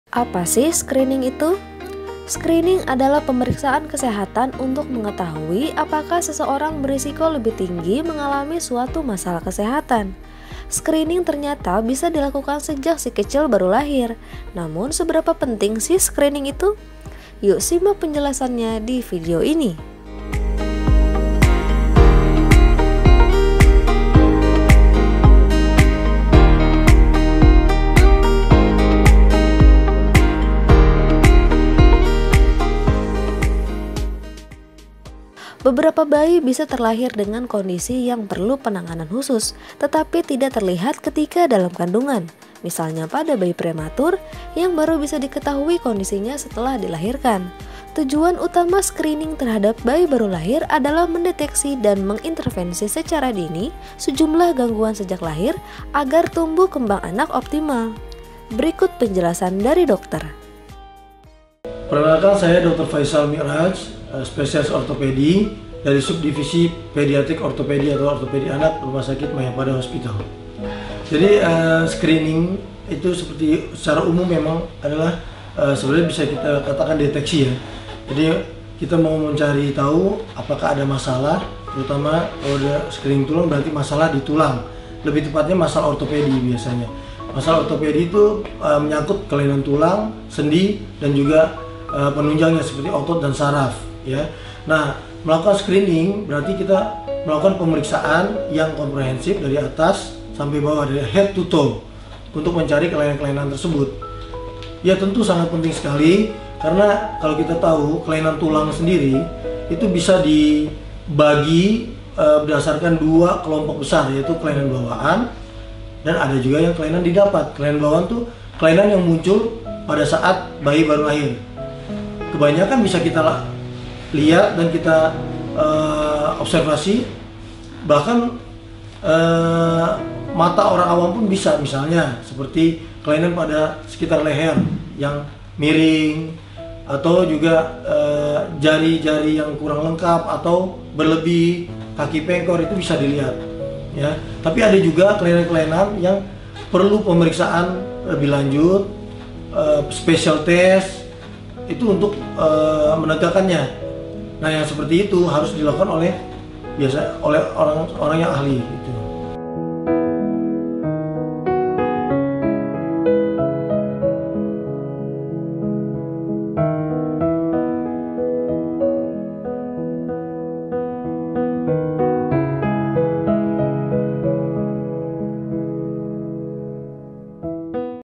Apa Itu Skrining Bayi Baru Lahir? Ini Alasan Skrining Penting Dilakukan!
Apa sih, skrining itu? Skrining adalah pemeriksaan kesehatan untuk mengetahui apakah seseorang berisiko lebih tinggi mengalami suatu masalah kesehatan.

Skrining ternyata bisa dilakukan sejak Si Kecil baru lahir. Namun seberapa penting sih, skrining itu? Yuk simak penjelasan di video ini!

00:02 Apa itu Skrining?
01:02 Pentingnya skrining pada bayi menurut pakar

EDITORIAL OFFICE

ADVERTISING